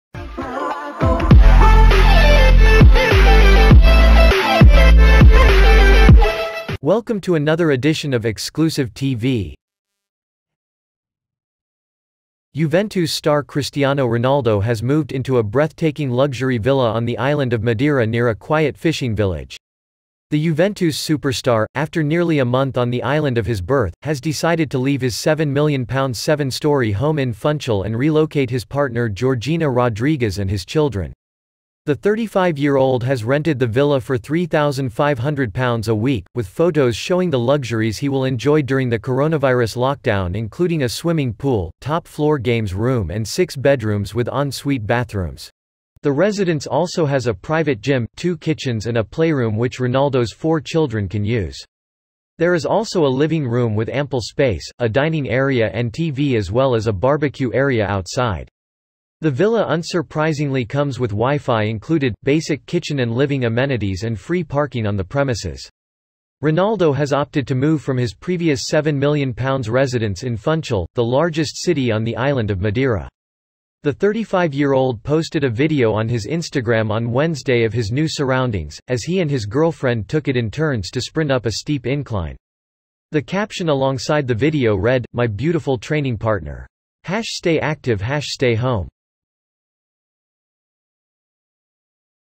INSIDE JUVENTUS STAR CRISTIANO RONALDO'S $4500USD LUXURIOUS VILLA
Cristiano Ronaldo has moved into a breathtaking luxury villa on the island of Madeira near a quiet fishing village.

The Juventus superstar, after nearly a month on the island of his birth, has decided to leave his £7m seven-storey home in Funchal and relocate his partner Georgina Rodriguez and his children